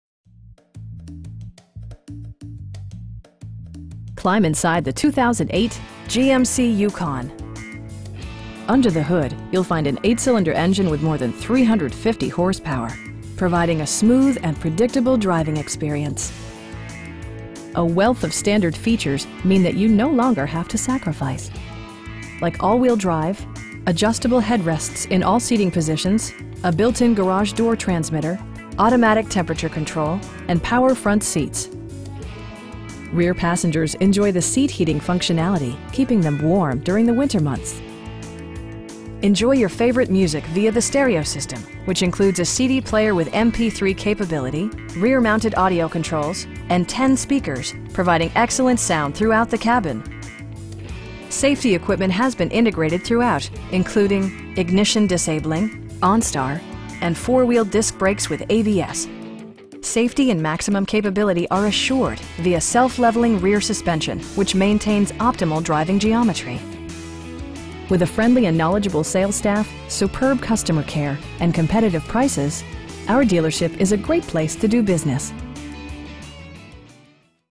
2008 GMC Yukon SUV in Chicago, IL 60616
Climb inside the 2008 GMC Yukon. Under the hood you'll find an 8 cylinder engine with more than 350 horsepower, providing a smooth and predictable driving experience. Well tuned suspension and stability control deliver a spirited, yet composed, ride and drive A wealth of standard features means that you no longer have to sacrifice. Like all wheel drive, adjustable headrests in all seating position a built-in garage door transmitter, automatic temperature control, rain sensing wipers, reverse sensing system, adjustable pedals, and power front seats. Rear passengers enjoy the seat heating functionality, keeping them warm during the winter months. Enjoy your favorite music via the stereo system, which includes a CD player with MP3 capability, rear mounted audio controls, steering wheel mounted audio controls, and 10 speakers, providing excellent sound throughout the cabin. Safety equipment has been integrated throughout, including: dual front impact airbags with occupant sensing airbag, head curtain airbags, traction control, ignition disabling, onStar, and 4 wheel disc brakes with ABS. Safety and maximum capability are assured via self leveling rear suspension, which maintains optimal driving geometry. It also arrives with a Carfax history report, indicating just one previous owner. With a friendly and knowledgeable sales staff, superb customer care, and competitive prices, our dealership is a great place to do business.